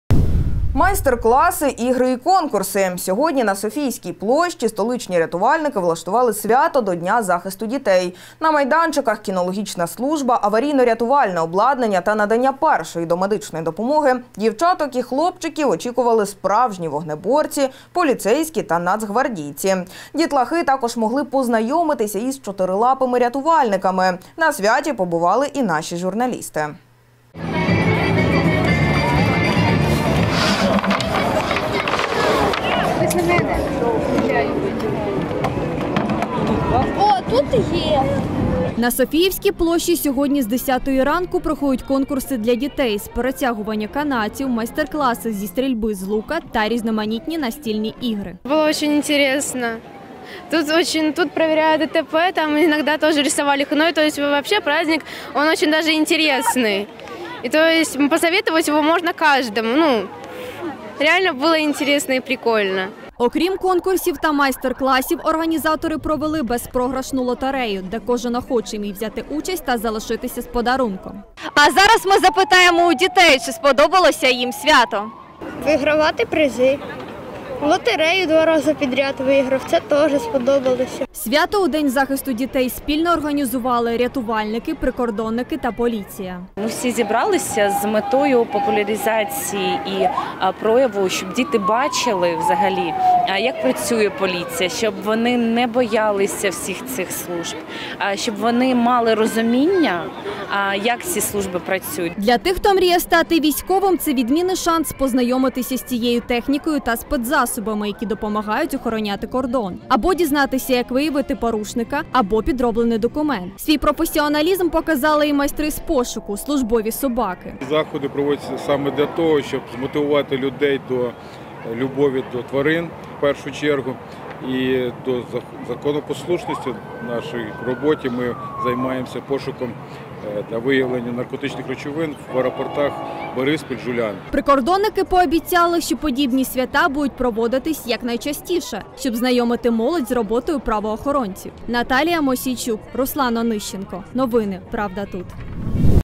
День захисту дітей
Літо розпочалося добрим і радісним святом – Міжнародним днем захисту дітей. На Софіївській площі відбулась зустріч дітей з прикордонниками. У дітей буде можливість взяти участь в різних іграх , майстер-класах ,конкурсах.